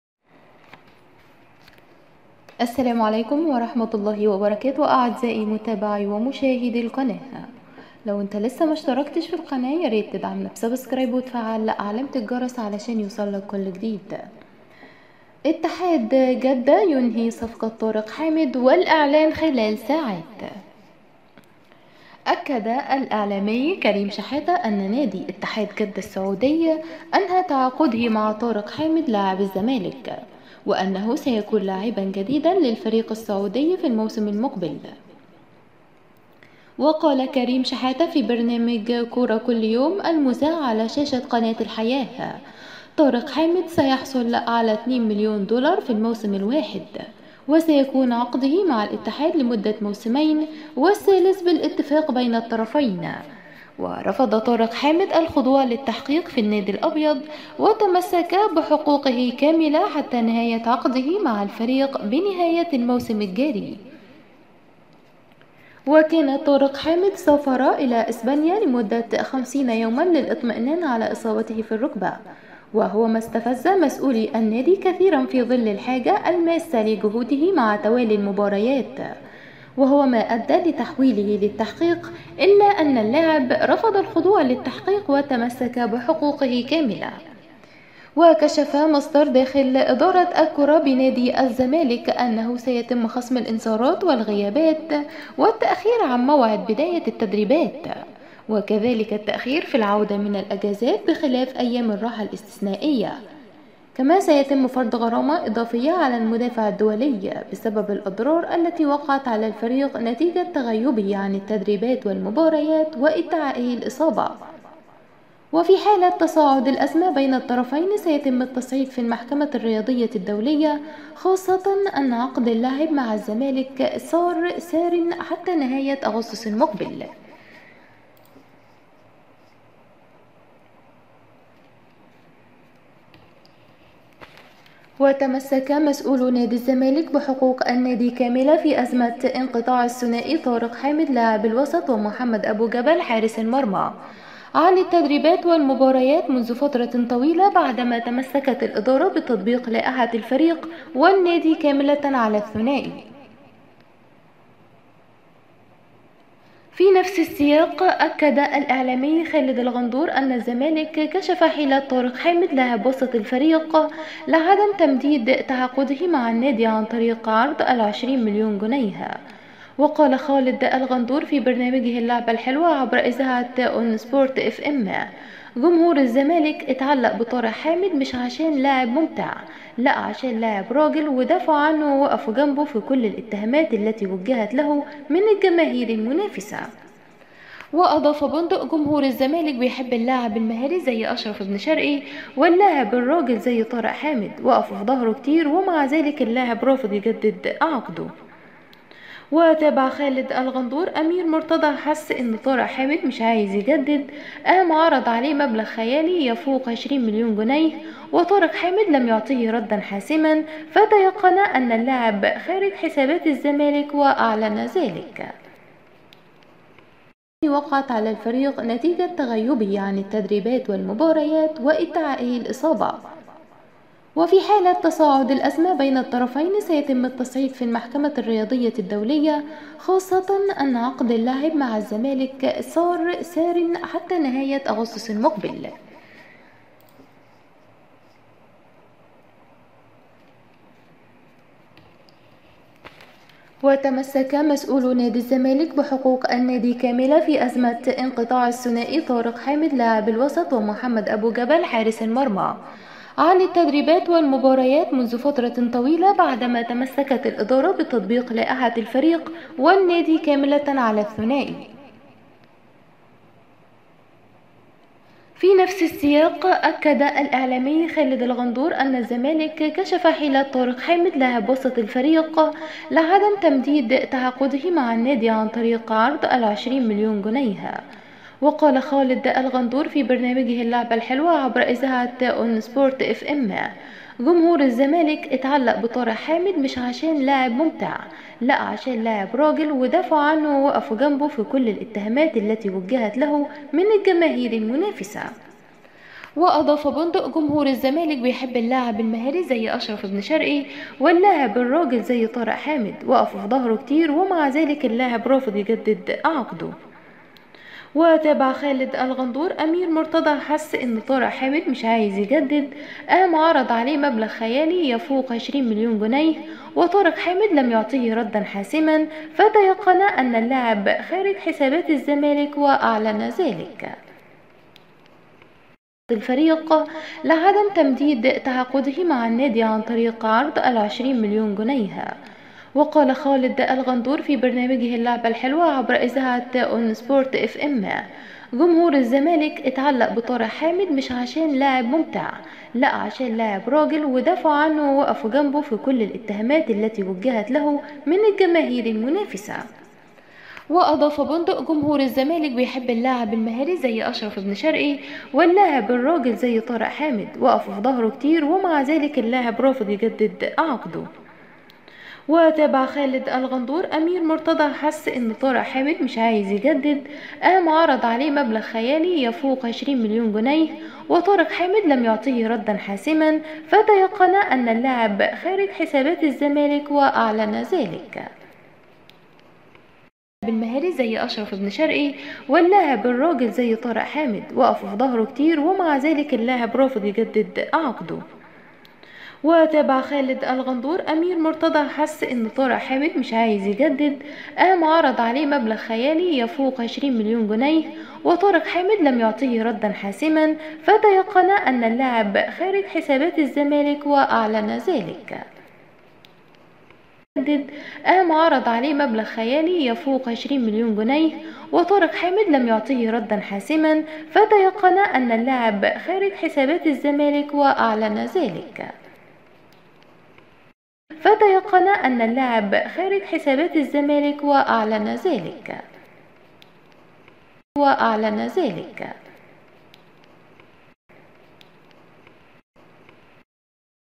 انهيار "مرتضي منصور" بعد اعلان اتحاد جدة رسميا التعاقد مع طارق حامد ويعلن خصم كل مستحقاته المتأخرة
الرجاء الإشتراك في القناة وتفعيل علامة الجرس ليصلكمالمزيد من الفيديوهات